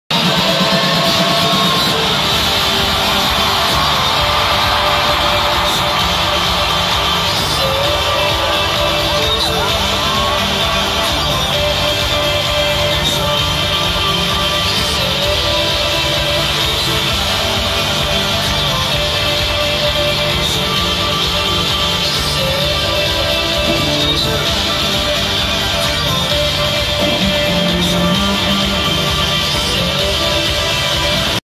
Coldplay
Live in SG. Uploading this clip instead of the 8-min one. Hotel internet can't seem to upload that 2GB file.